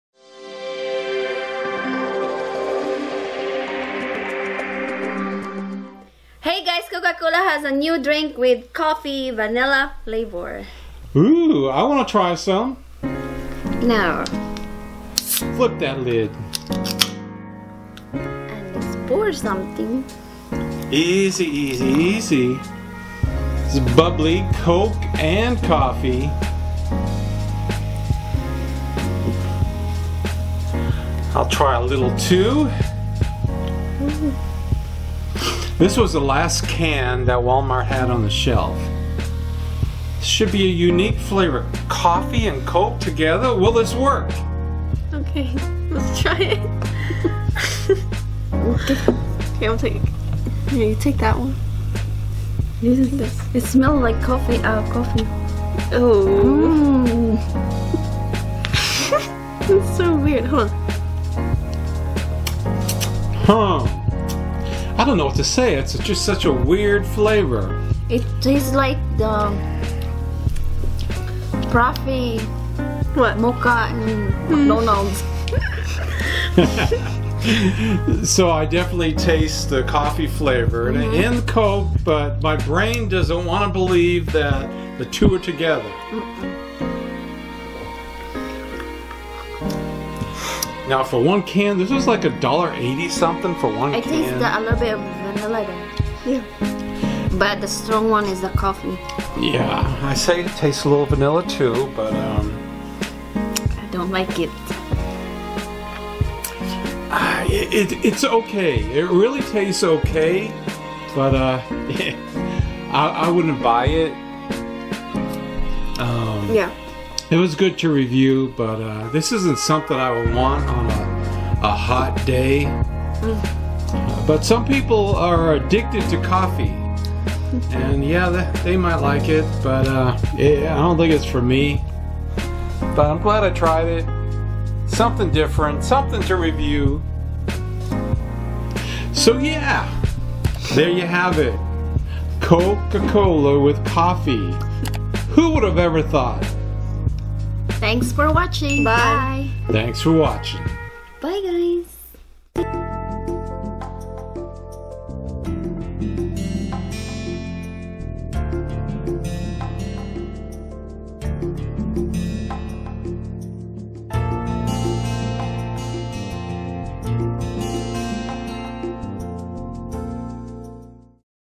Coca Cola with Coffee
Coca Cola has a new limited edition flavor and we thought we'd give it a try!